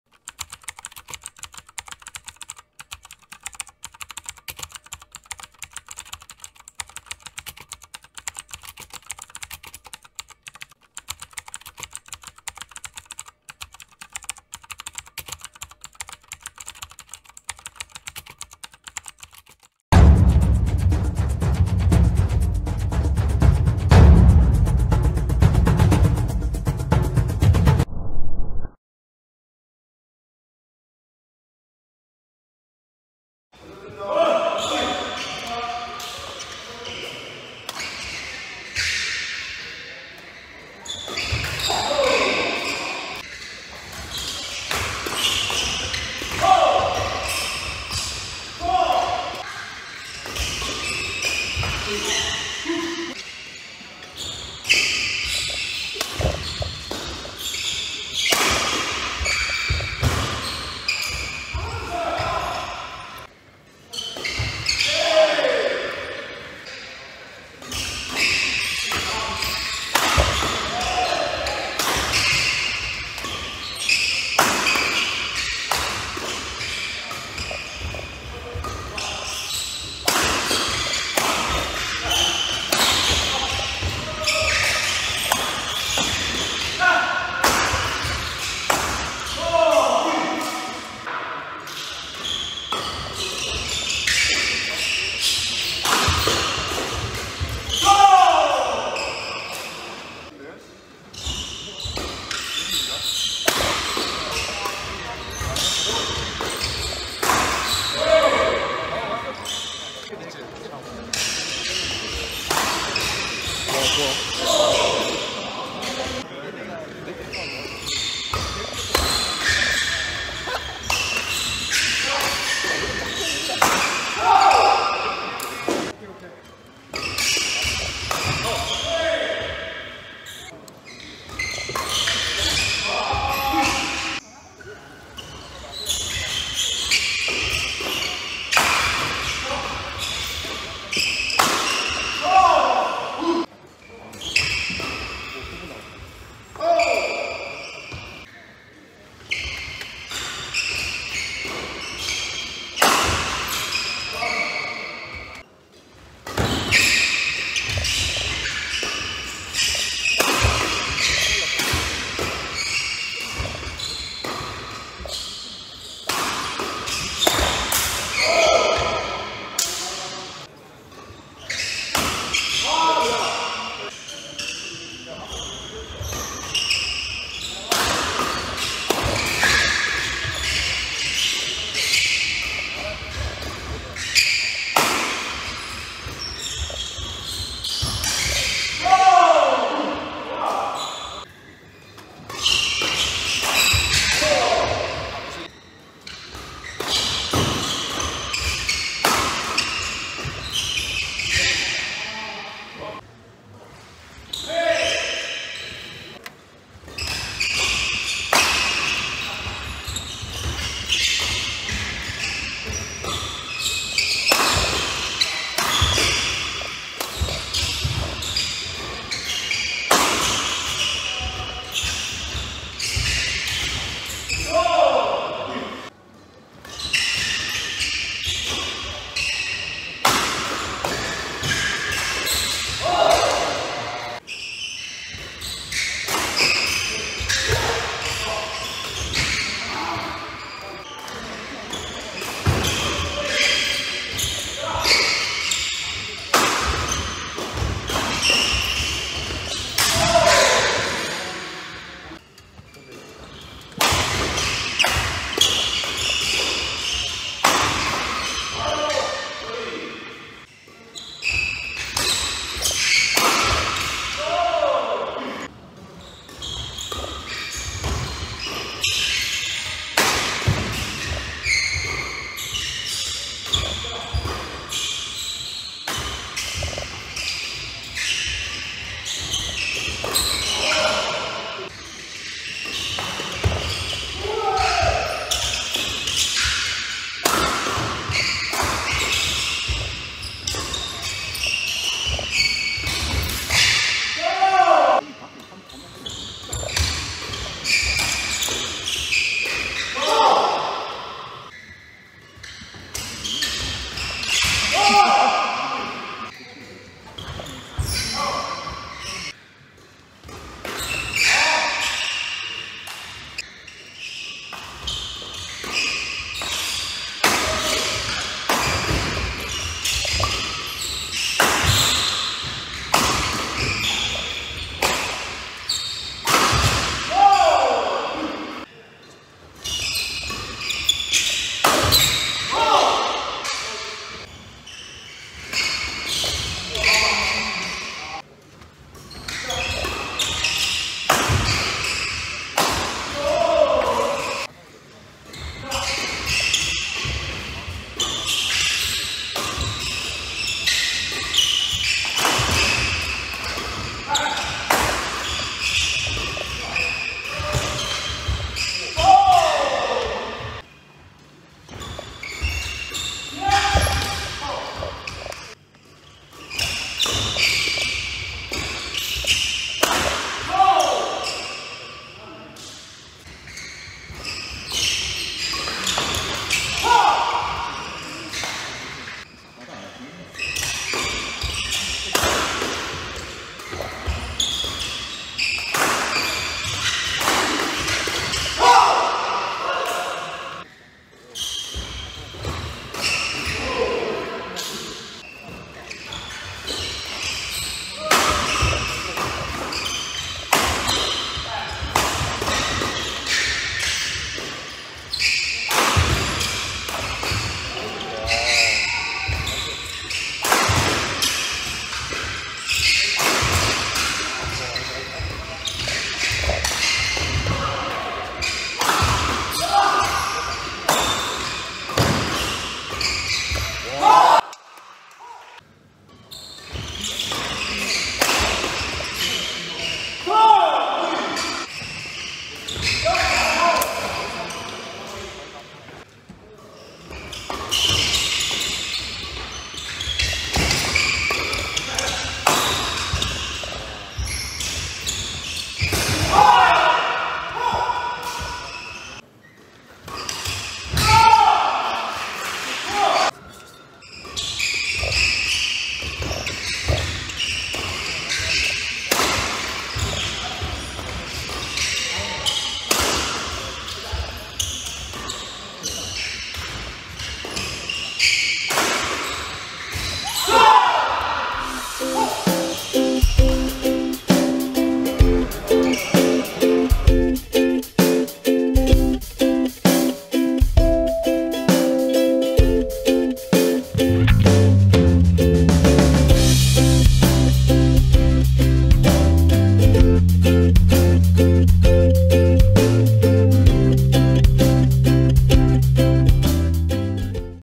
3조1경기(TEAM AXTHION)김봉준.이준혁 vs (슈프림&미카즈)정유석.황준원
제1회 정민턴배 전국 동호인 남복 최강전
예선 3조 1경기! (TEAM AXTHION)김봉준.이준혁 vs (슈프림&미카즈)정유석.황준원

1등 상금 50만원(2등 30만원) 에 도전하는 전국 동호인들의 치열한 경기!!
과연 누가 우승할 것인가?